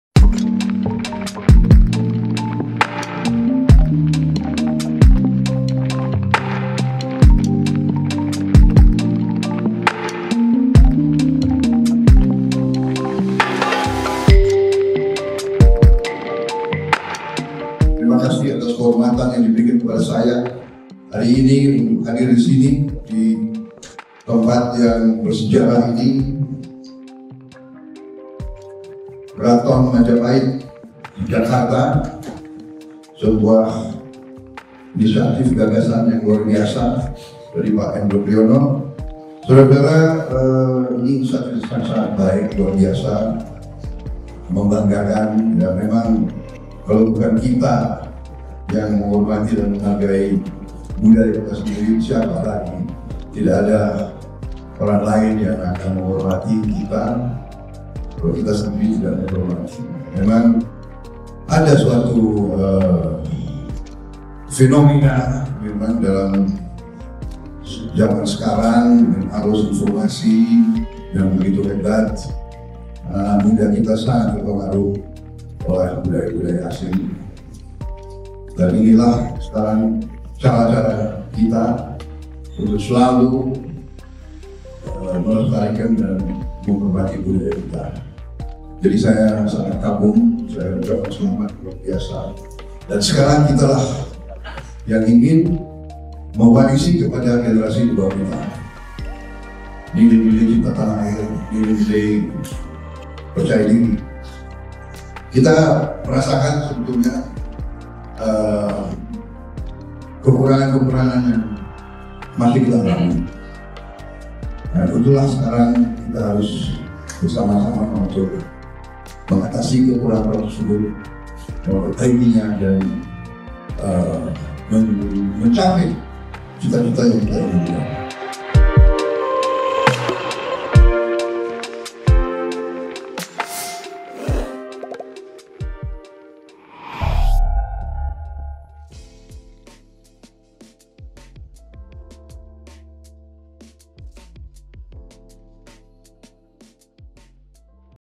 Jadi Sorotan, Momen Haru Prabowo Gandeng Erat Tangan Jenderal Sepuh Try Sutrisno
MERDEKA.COM - Menteri Pertahanan sekaligus Presiden terpilih RI Prabowo Subianto meresmikan replika Istana Majapahit. Menurutnya, ini merupakan bagian dari upaya melestarikan budaya dan sejarah bangsa.

Pada kesempatan tersebut, turut hadir antara lain Wakil Presiden Ke-6 RI Try Sutrisno, mantan Kepala BIN A.M Hendropriyono, Kepala Staf Kepresidenan Moeldoko, Menko PMK Muhadjir Effendy, Menkominfo Budi Arie Setiadi, Menteri Perdagangan Zulkifli Hasan, Kapolri Jenderal Listyo Sigit Prabowo, Kepala Staf TNI Angkatan Laut (KSAL) Laksamana TNI Muhammad Ali, Ketua Dewan Pertimbangan Presiden Wiranto, Staf Khusus Presiden Diaz Hendropriyono.

Hadir pula mantan Menteri Perhubungan Agum Gumelar, mantan Menteri Tenaga Kerja Abdul Latief, mantan Panglima TNI Jenderal TNI (Purn) Andika Perkasa, mantan Ketua Mahkamah Agung Bagir Manan, Mantan Ketua Mahkamah Konsitusi Jimly Asshiddiqie, hingga mantan Menko Perekonomian Chairul Tanjung. (Bimo / Ester)